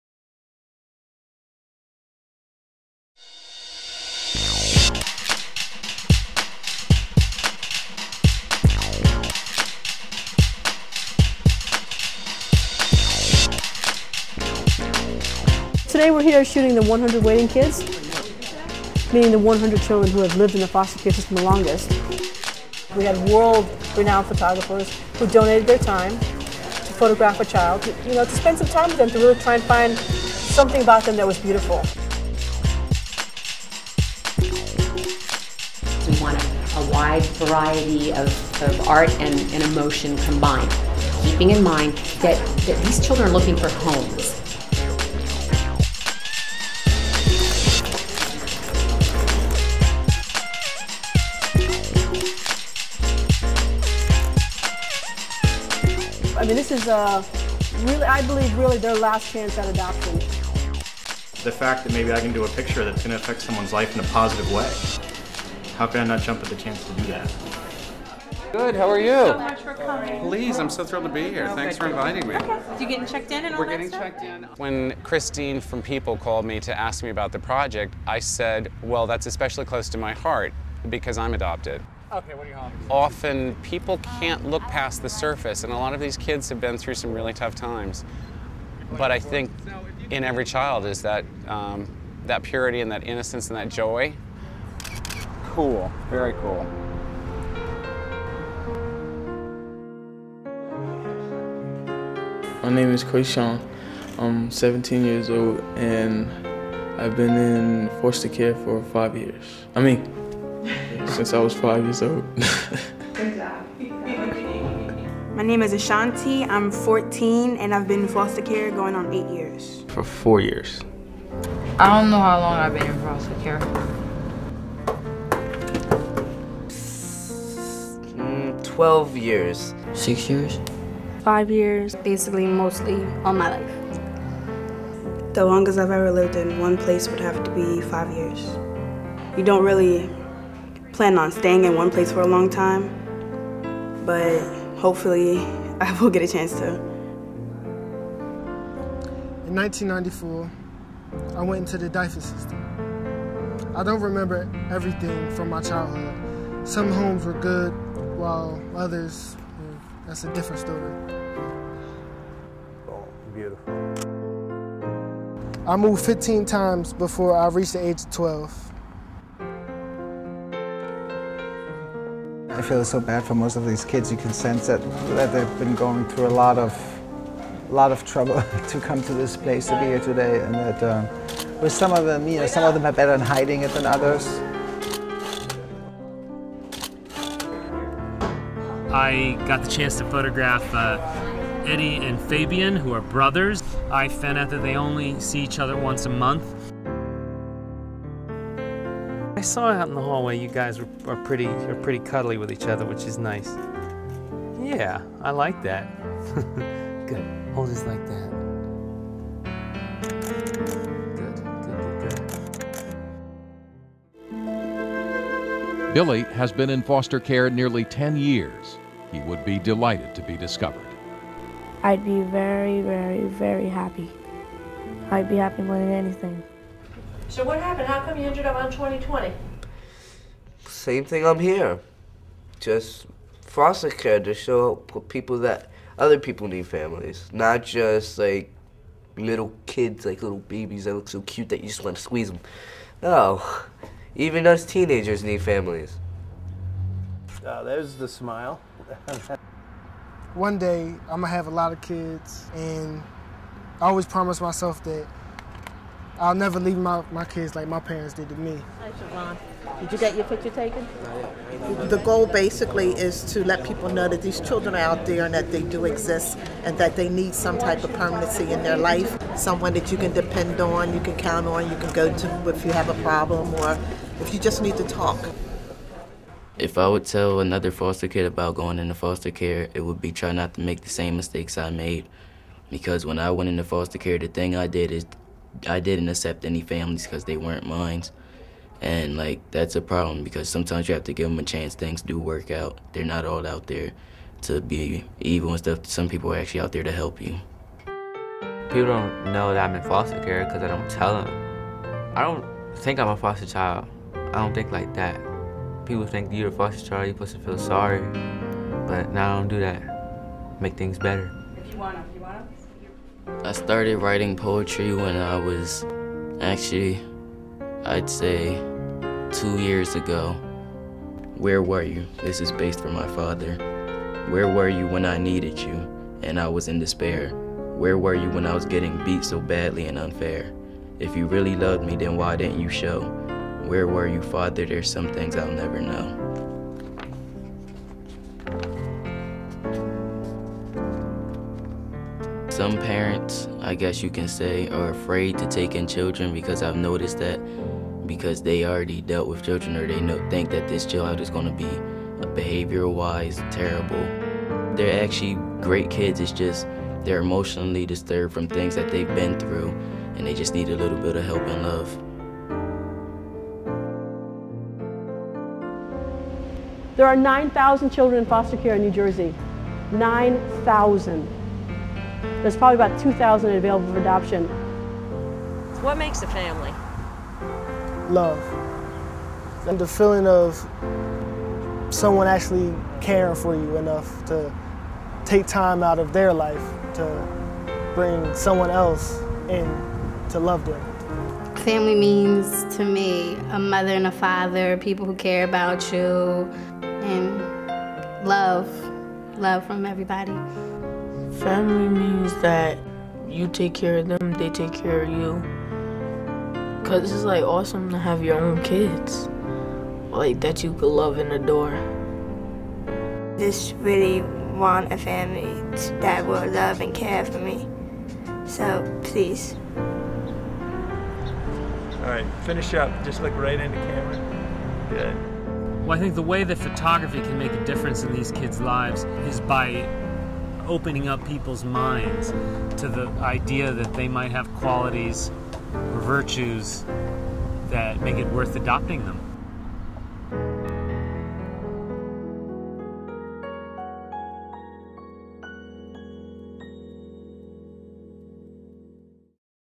100 Waiting Children
Thousands of children spend years of their lives in the foster care system without ever finding a permanent home. The Heart Gallery of New Jersey is pursuing an innovative way of connecting these kids with potential families. Nationally acclaimed photographers volunteered to take portraits of the 100 children who have been in foster care the longest in the state. Through these compelling portraits and major media exposure, the Heart Gallery hopes to help place these kids in loving, stable homes. In 2005, when the Heart Gallery of New Jersey was launched, the organization was instrumental in helping over 100 children get adopted. Talking Eyes Media was responsible for all multi-media aspects of the "100 Waiting Children" campaign.